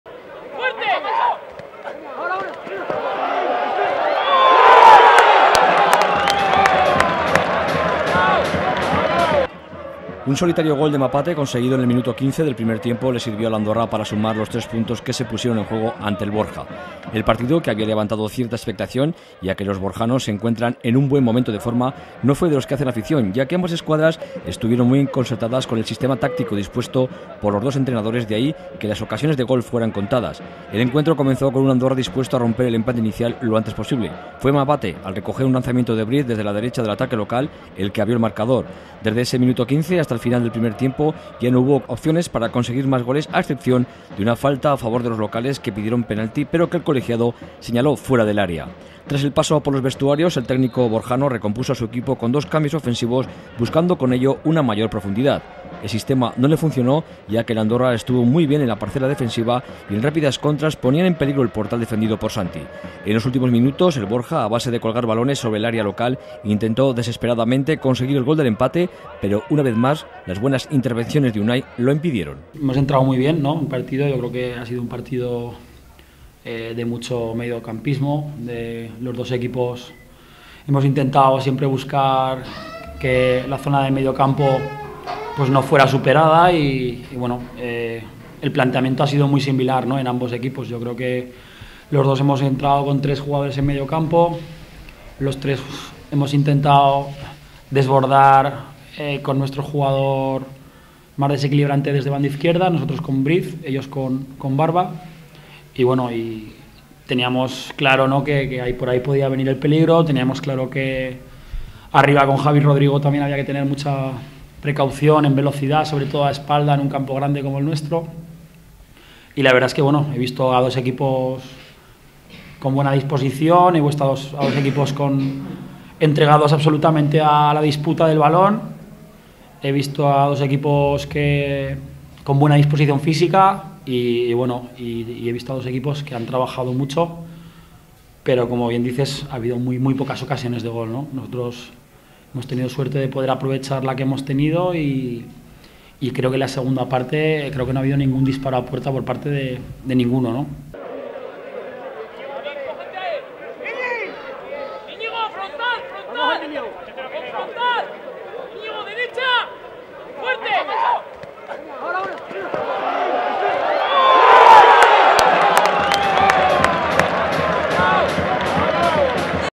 La Comarca.tv - Fútbol Andorra 1 Borja 0
Victoria del Andorra frente al Borja por un gol a cero en el partido disputado en el Endeiza de la villa minera. Mapate consiguió el único gol del choque en un partido intenso y muy defensivo que tuvo muy pocos tiros a puerta.